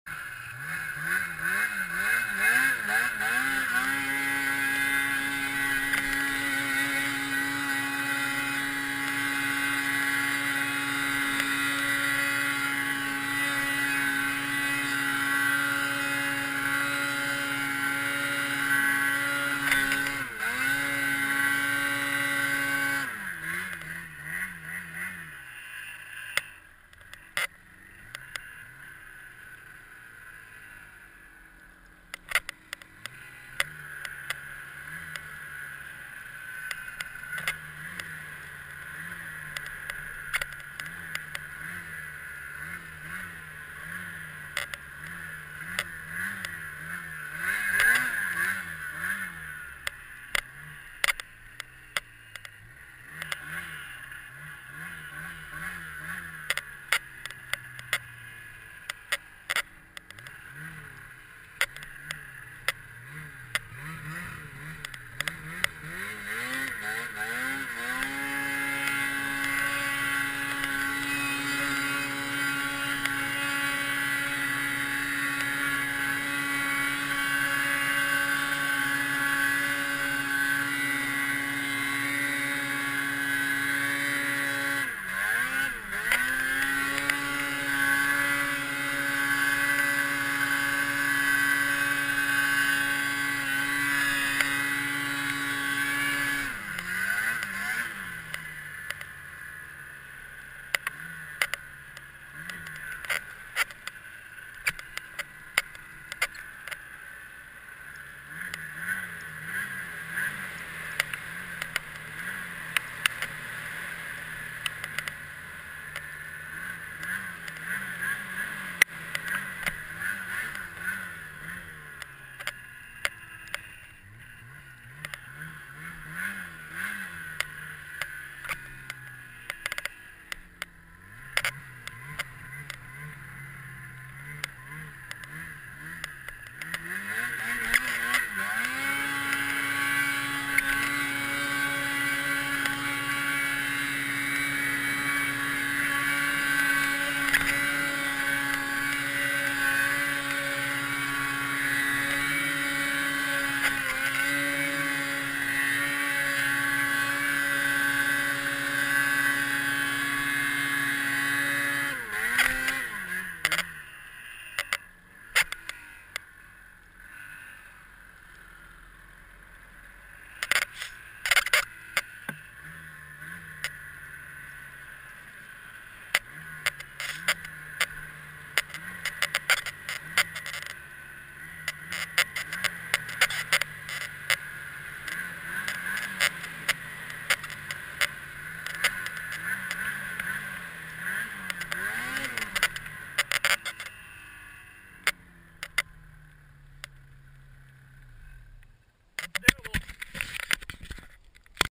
RK Tek XP 860 pulling a virgin chute. No fancy music or anything.. just RAW HP and DEEP POWDER...
Temp that day was -25F.. BRUTALLY COLD!!